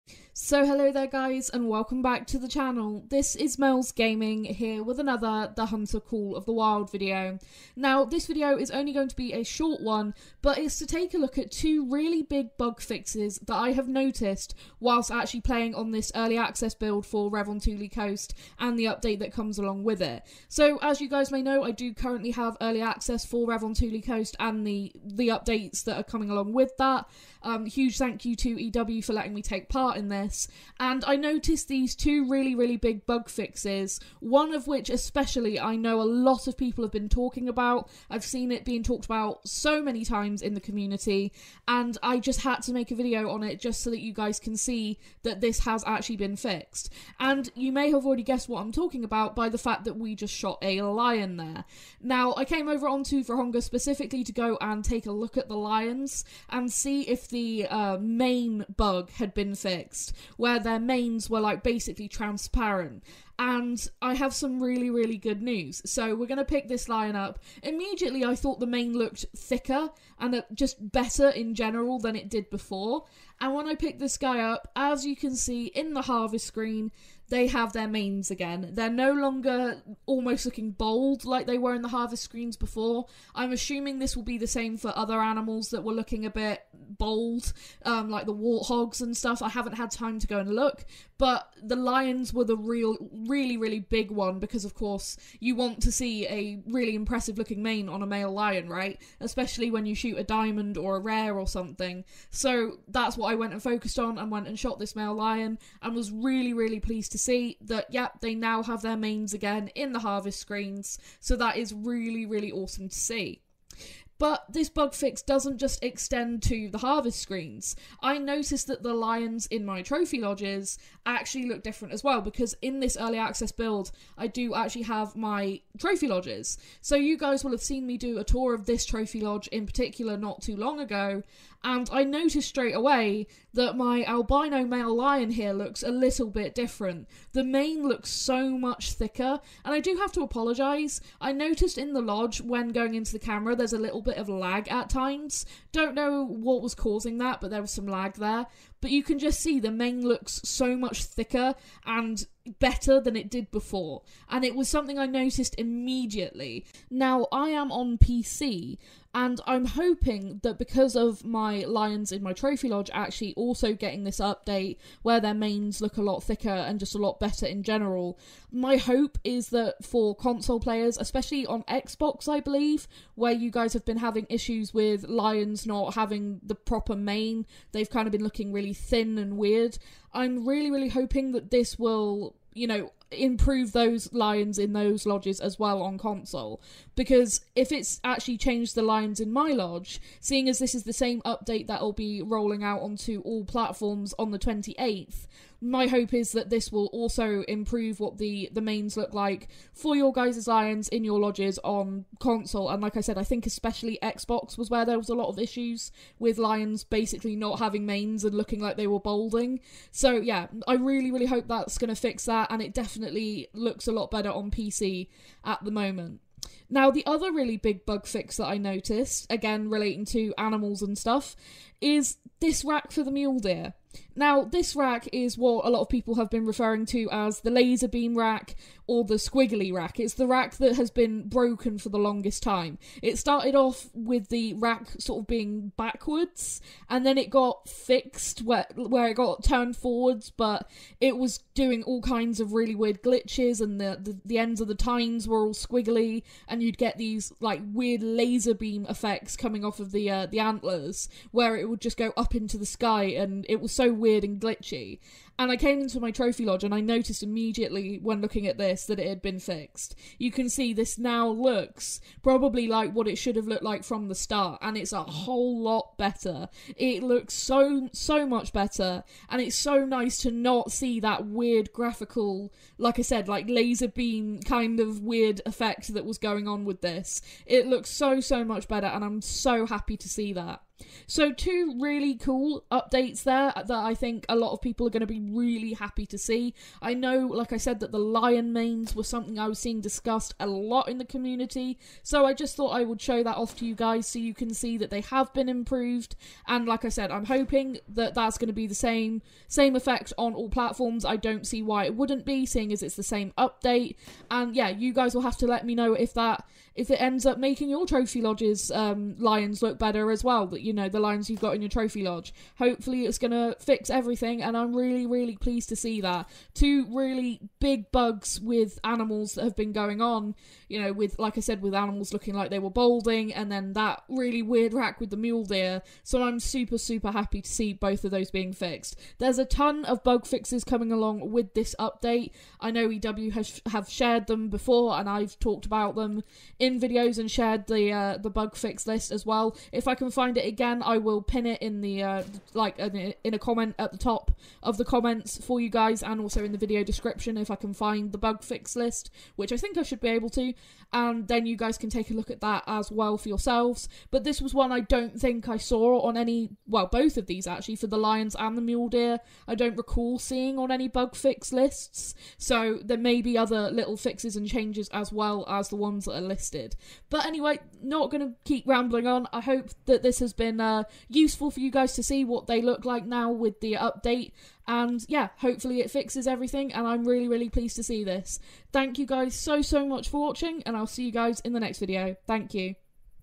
LION MANES ARE FIXED, AND THE BROKEN LVL 5 MULE DEER RACK IS FIXED TOO WITH THE REVONTULI UPDATE!
As promised, the preliminary patch notes for the Revontuli Coast update!